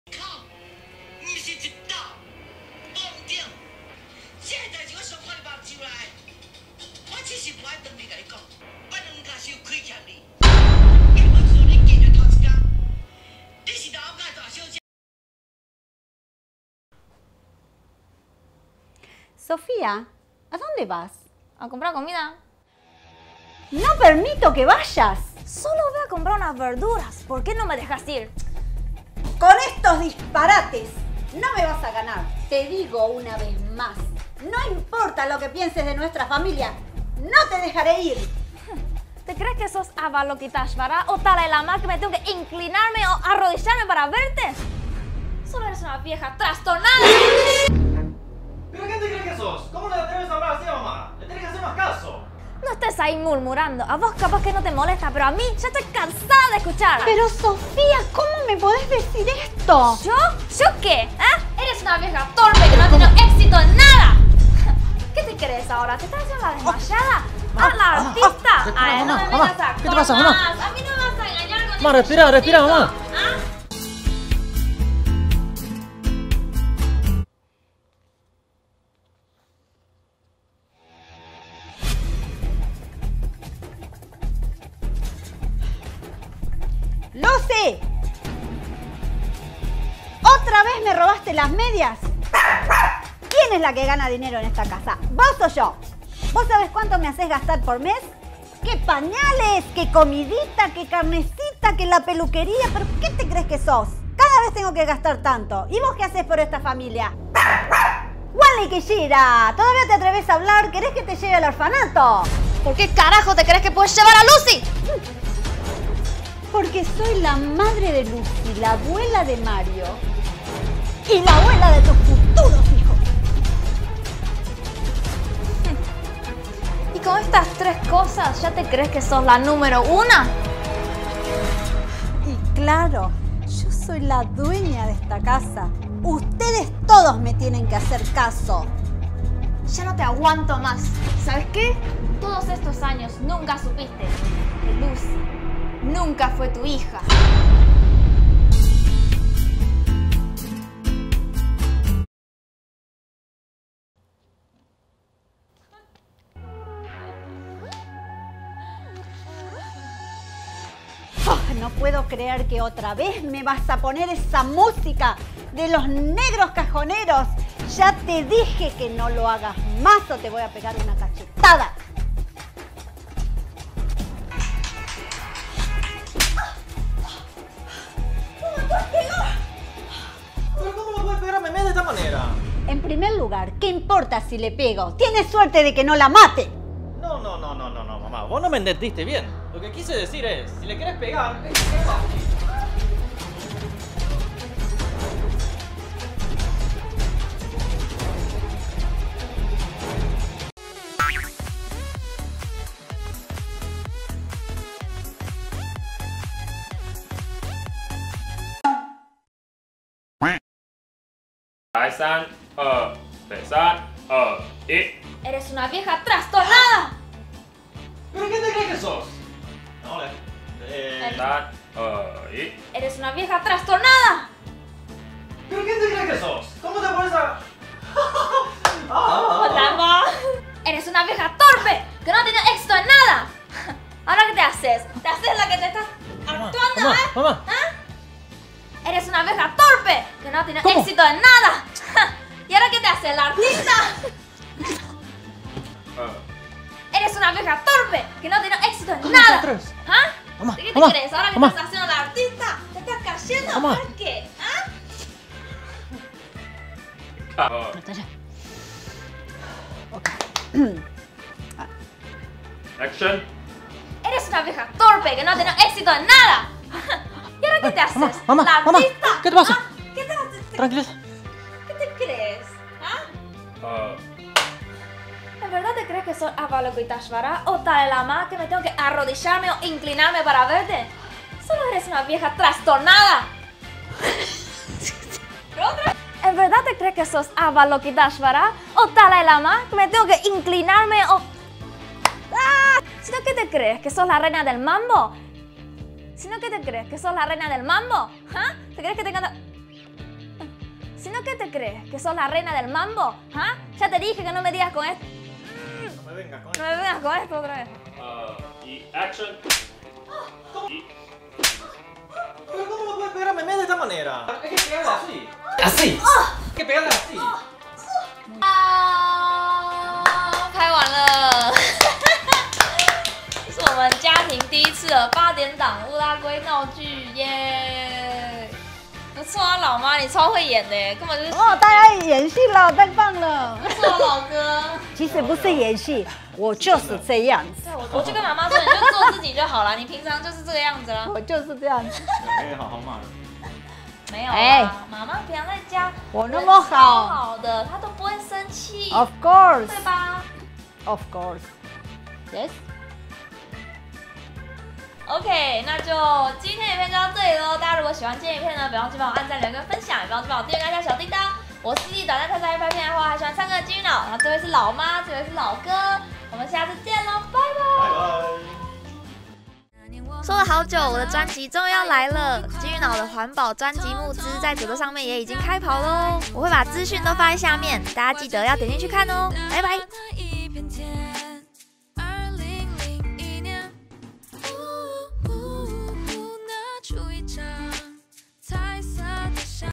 如果八點檔發生在烏拉圭家庭｜金魚腦 Goldfish Brain feat. @-PattySantoro
如果八點檔發生在烏拉圭家庭｜金魚腦 Goldfish Brain feat.  @派蒂·三多羅Patty Santoro

人生如戲，戲如人生。
關於金魚家的愛恨糾葛一觸即發...
究竟金魚腦能夠化解和母親的心結，擁抱親情嗎？

當夢想跟理想可以一起完成時，就是最幸福的時候了🥰
我的夢想是有張自己的專輯🎧我的理想是能讓更多人接觸環保議題♻️
但我的力量還是很有限，如果你願意支持我的話，請加入這次的嘖嘖募資計畫❤️

・Uber百元折價序號：yz9v2616ue